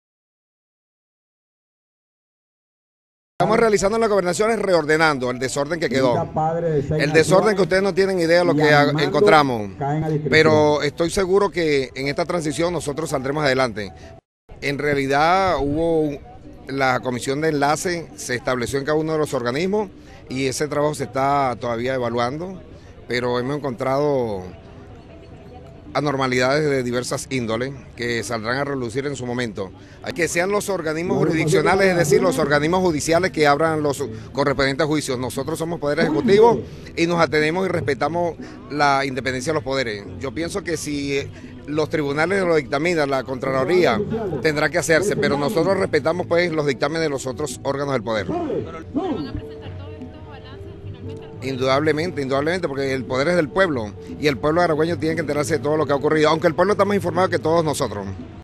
Gestión saliente dejo muchas irregularidades
El secretario de gobierno del estado Aragua, José Villarroel, aseguró que la gestión saliente dejó grandes irregularidades que serán elevadas a los órganos competentes, que serán los encargados de sancionar a quienes hayan cometido actos ilícitos.